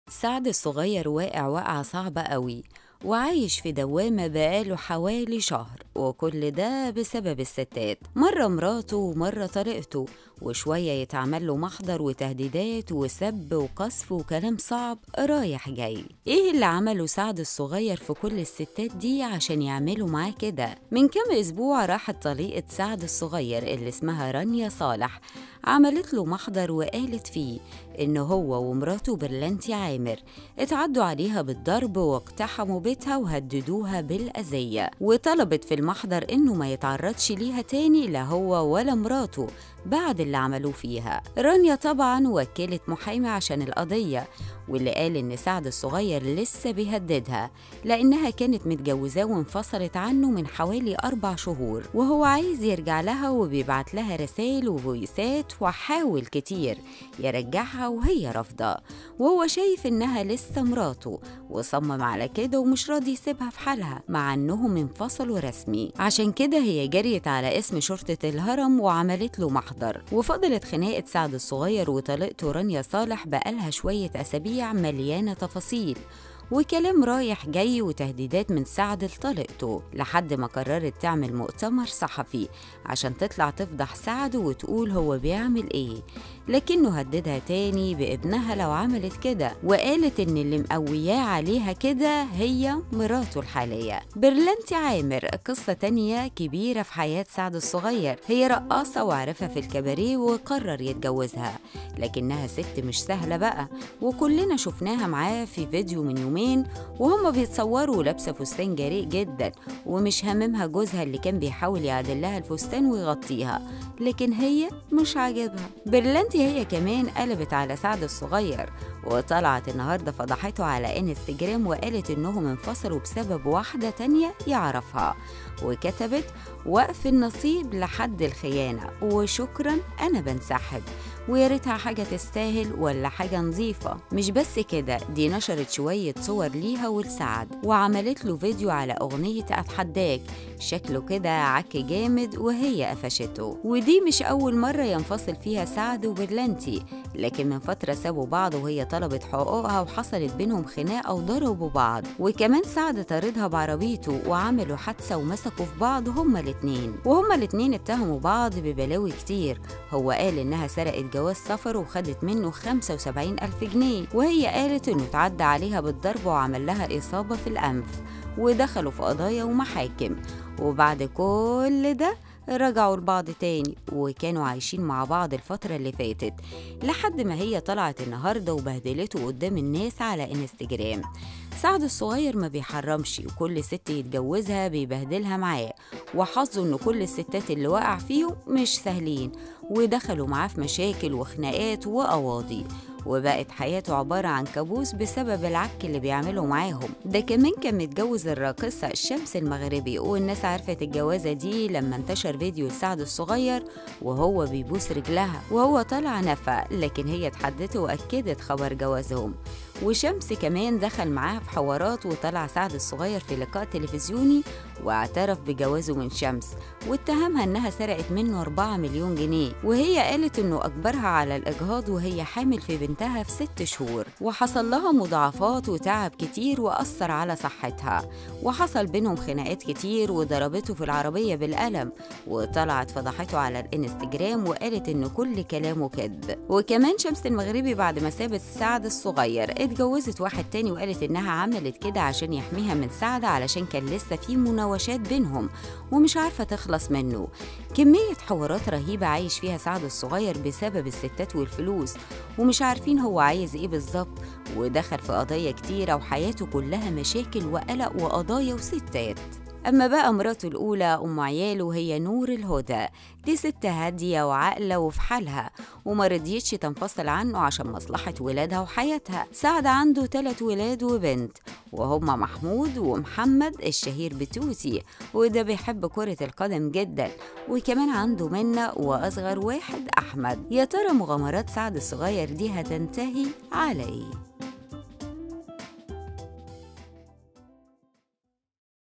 برلنتي فضحته ورانيا عملتله محضر والراقصة شمس خدت منه 4 مليون جنيه .. 3 ستات مدوخين سعد الصغير
سعد الصغير واقع وقعة صعبة اوي وعايش في دوامة بقاله حوالي شهر، وكل ده بسبب الستات ، مرة مراته ومرة طليقته ، وشوية يتعمله محضر، وتهديدات وسب وقذف وكلام صعب رايح جاي .. ايه اللي عمله سعد الصغير في كل الستات دي عشان يعملوا معاه كدا ؟